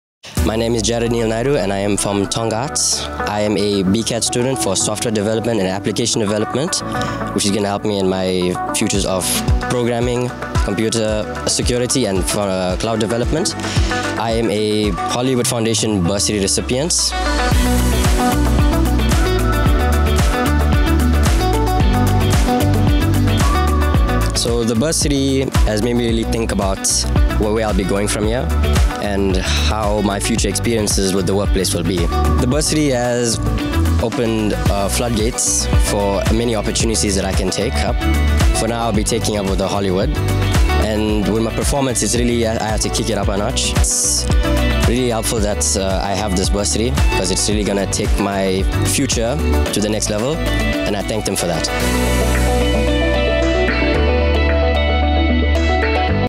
Hollywood Foundation Bursary Recipient | Jarod Naidoo
Meet Jarod Naidoo, one of our Hollywood Foundation Bursary recipients. Banele shares his personal journey and the positive changes he's experienced since receiving the bursary💜
If Jarod’s story inspires you and you're interested in applying for a bursary,
T&Cs apply.